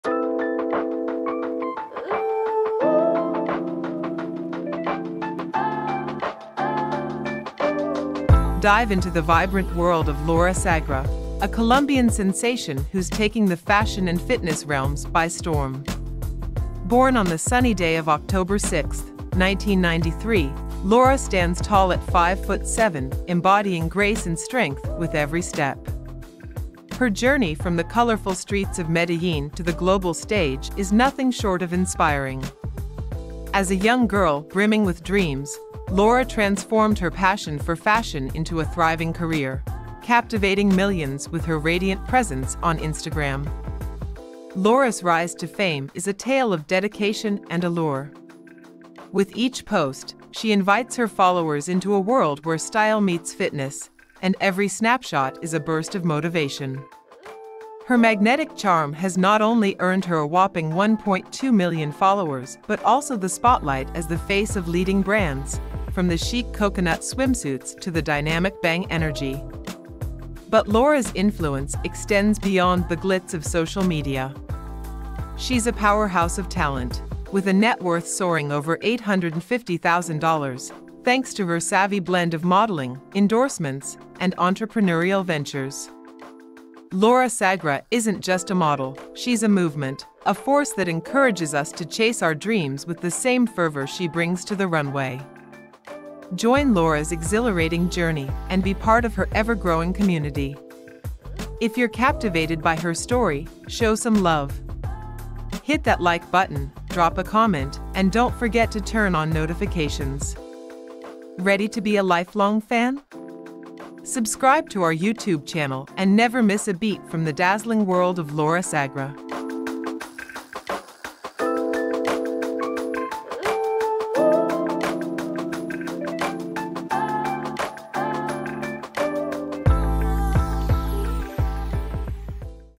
Laura Sagra: Plus Size Model, Bio, Body Measurements, Age, Height, Weight, Net Worth
Discover Laura Sagra, the vibrant plus-size model! Learn about her bio, body measurements, age, height, weight, and net worth in this fun-filled video.